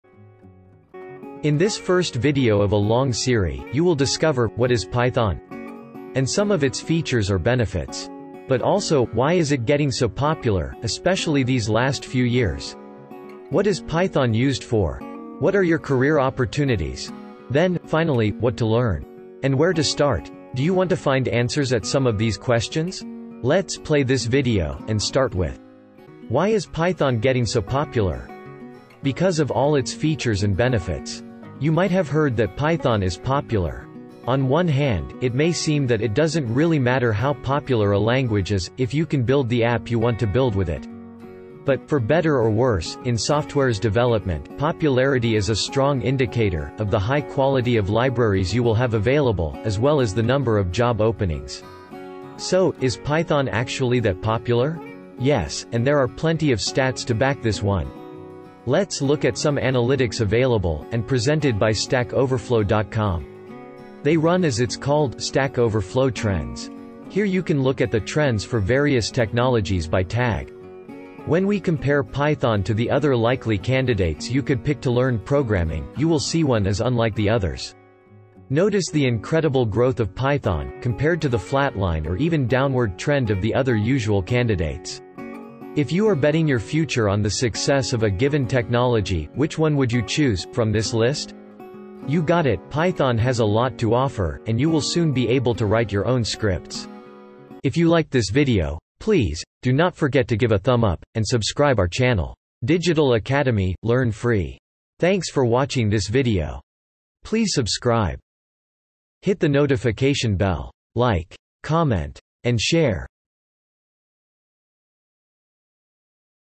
Why is Python Getting so Popular? (Features & Trends) - Python Tutorial for Beginners
🖥️  Why is Python getting so popular?

Because of all its features and benefits, You might have heard that Python is popular. On one hand, it may seem that it doesn’t really matter how popular a language is, if you can build the app you want to build with it.

But, for better or worse, in softwares development, popularity is a strong indicator, of the high quality of libraries you will have available - as well as the number of job openings.

So, is Python actually that popular? YES, and there are plenty of stats to back this one. Let’s look at some analytics available, and presented by StackOverflow. They run as its called “StackOverflow Trends”. There, you can look at the trends for various technologies by Tag. When we compare Python to the other likely candidates you could pick to learn programming, you will see one is unlike the others.

Notice the incredible growth of Python, compared to the flatline or even downward trend of the other usual candidates. If you are betting your future on the success of a given technology, which one would you choose from this list? You got it, Python has a lot to offer, and you will soon be able to write your own scripts!

- Digital Academy™ 🎓